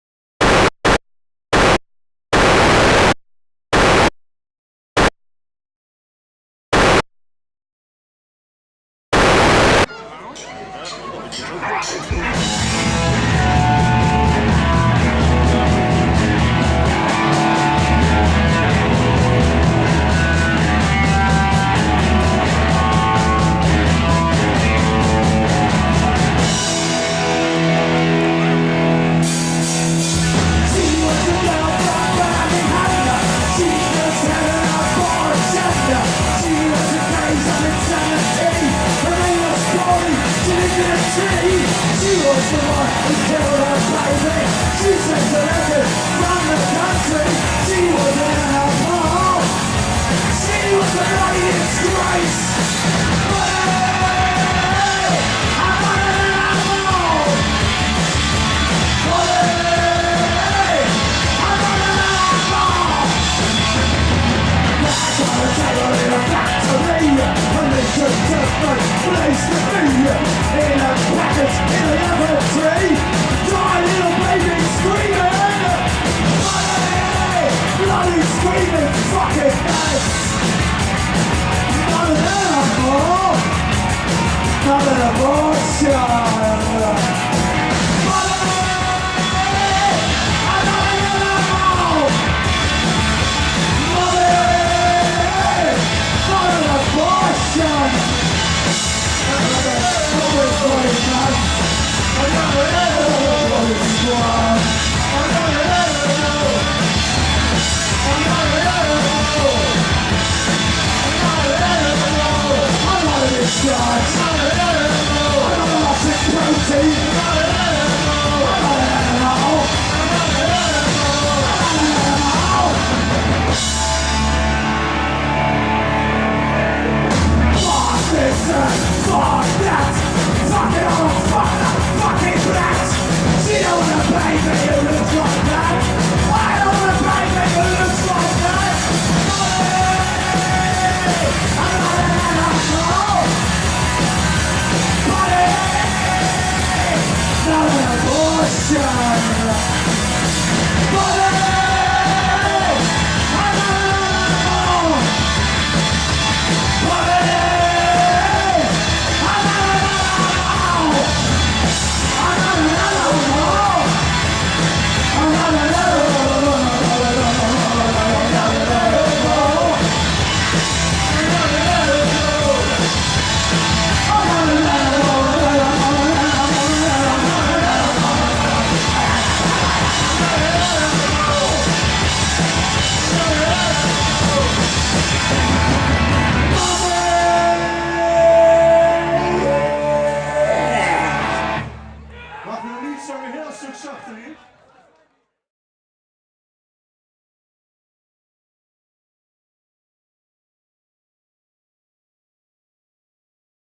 Kaaspop 2015-The Stiff Bodies
Optreden van the Stiff op de Beestenmarkt.